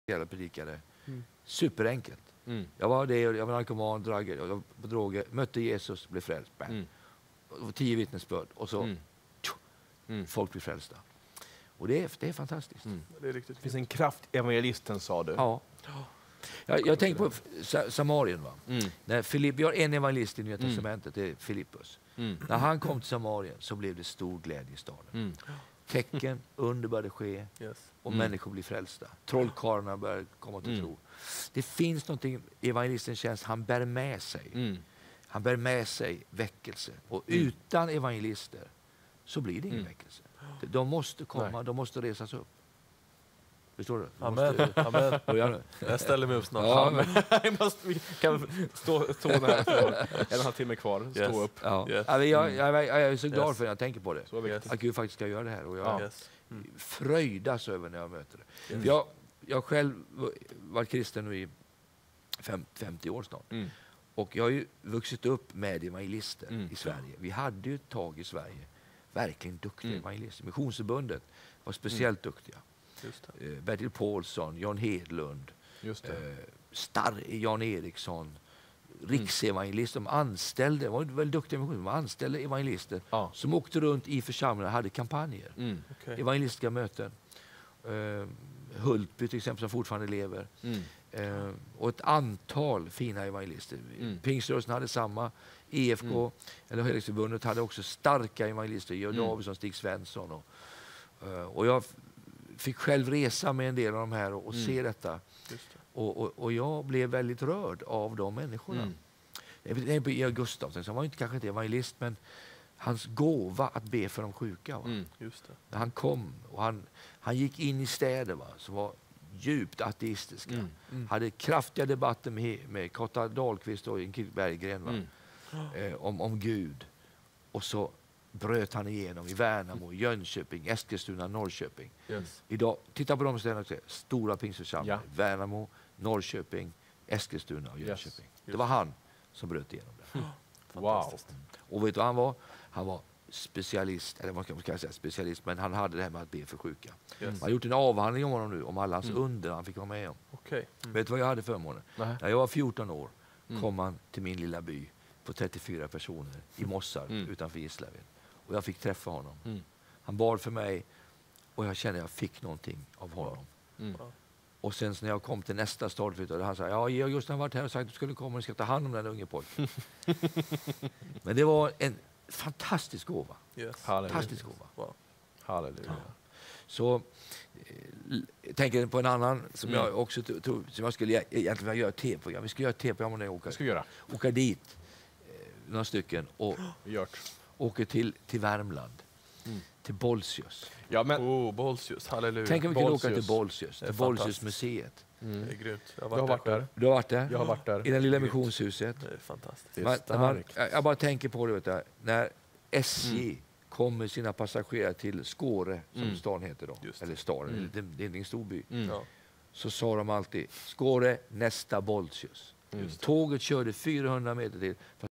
Evangelisterna förr i Sverige
Evangelisterna från förr i Sverige förvandlade vårt land! Vi behöver ge plats för dem igen!